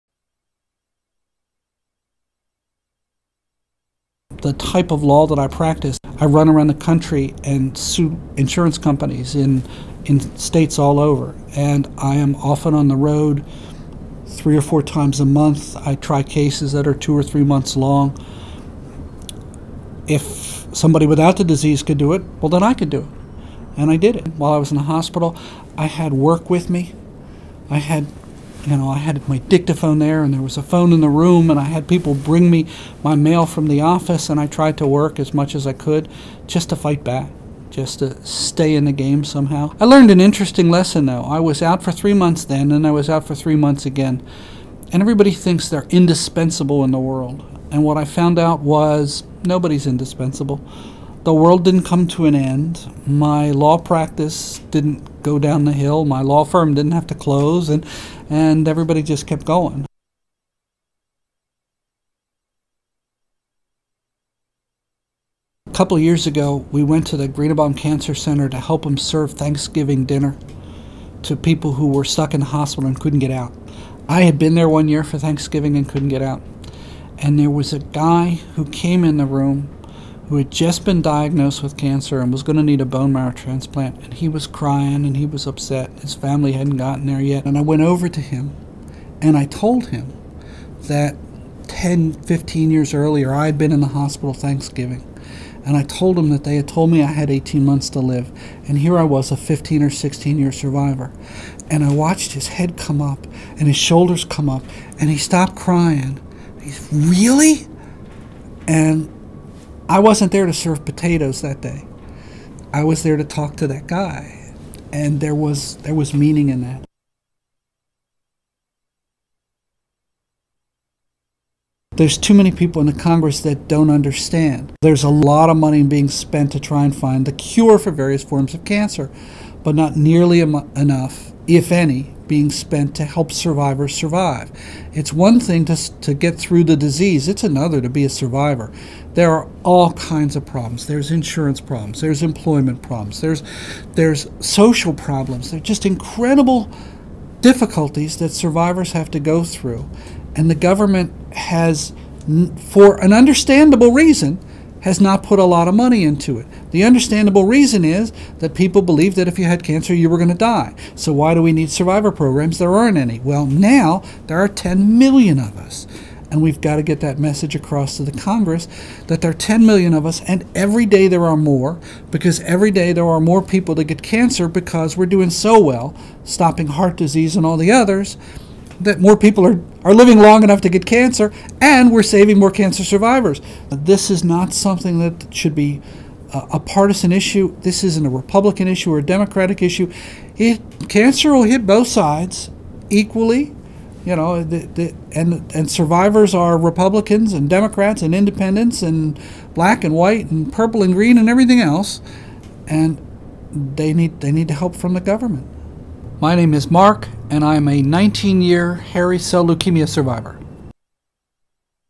Survivor Interview Mark K.
Mark is a hairy cell leukemia survivor.  He talks about experiencing the aftereffects of cancer treatment, managing a compromised immune system, and participating in LIVESTRONGTM Day.